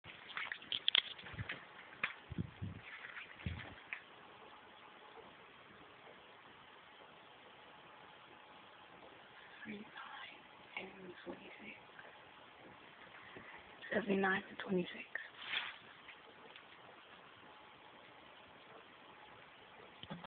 79 - 26 crash
i camp in beging then i ran around they kept spawning i just killed them